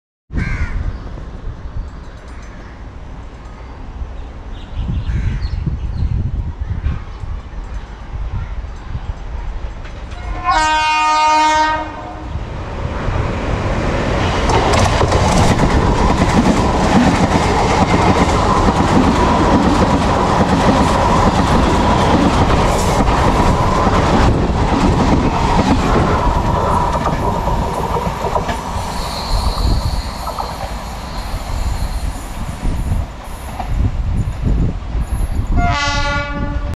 Fast Local Train Speeding through the Railway Bridge | Indian Railways
Dear Friends Watch the Super Fast Mumbai Local Train Speeding through the Beautiful Railway Bridge. Indian Railways @MyTrainsINDIA